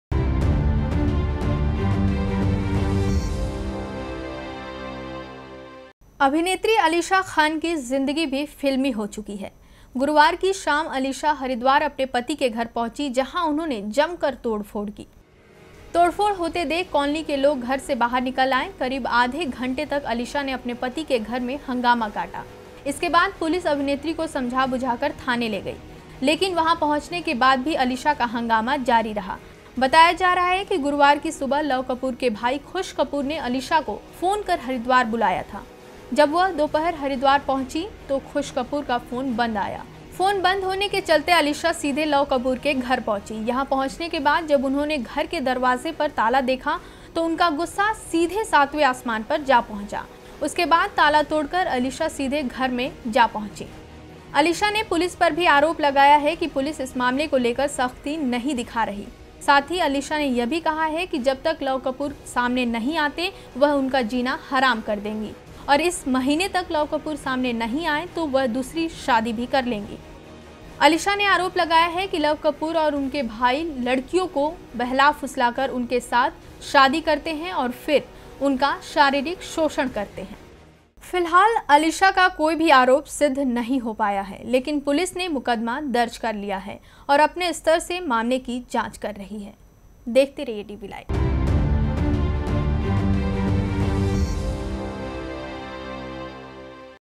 DB LIVE | 26 AUGUST 2016 | Actress Alisha Khan creates drama at in-laws' house
अभिनेत्री अलीशा खान की ज़िदगी भी फिल्मी हो चुकी है। गुरुवार की शाम अलीशा हरिद्वार अपने पति के घर पहुंची जहां उन्होंने जमकर तोड़फोड़ की। तोड़फोड़ होते देख कॉलोनी के लोग घर से बाहर निकल आए। करीब आधे घंटे तक अलीशा ने अपने पति के घर में हंगामा काटा। इसके बाद पुलिस अभिनेत्री को समझा बुझाकर थाने ले गई। लेकिन वहां पहुंचने के बाद भी अलीशा का हंगामा जारी रहा। बताया जा रहा है कि गुरुवार की सुबह लव कपूर के भाई खुश कपूर ने अलीशा को फोन कर हरिद्वार बुलाया था। जब वह दोपहर बाद हरिद्वार पहुंची तो खुश कपूर का फोन बंद आया। फोन बंद होने के चलते अलीशा सीधे लव कपूर के घर पहुंची। यहां पहुंचने के बाद जब उन्होंने घर के दरवाज़े पर ताला लटका देखा तो उनका गुस्सा सीधे सातवें आसमान पर जा पहुंचा। उसके बाद ताला तोड़कर अलीशा सीधे घर में पहुंच गई। अलीशा ने पुलिस पर भी आरोप लगाया है कि पुलिस इस मामले को लेकर सख्ती नहीं दिखा रही। साथ ही अलीशा ने यह भी कहा कि जब तक लव कपूर सामने नहीं आते वह उनका जीना हराम कर देंगी और इस महीने लव कपूर सामने नहीं आए तो  वह दूसरी शादी भी जल्द कर लेंगी। अलीशा ने आरोप लगाया कि लव कपूर और उनके भाई लड़कियों को बहला-फुसलाकर उनके साथ शादी करते हैं और फिर उनका शारीरिक शोषण करते हैं।